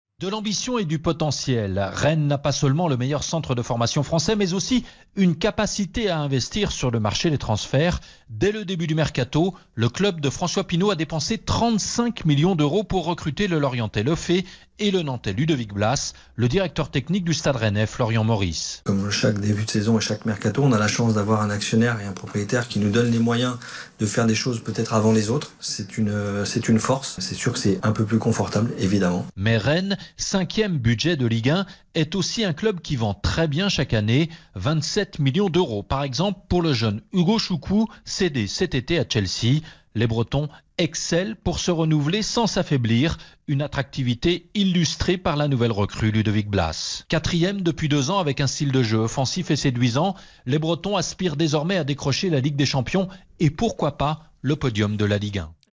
Reprise de la Ligue 1 : Rennes
⚽ Ce vendredi marque le lancement de la saison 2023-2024 du championnat de France de Ligue 1. L'occasion de faire le point sur la forme, les effectifs et les ambitions de cinq clubs importants de notre championnat. Aujourd'hui, direction la Bretagne avec le Stade Rennais qui compte bien s'installer sur le podium. 🔴⚫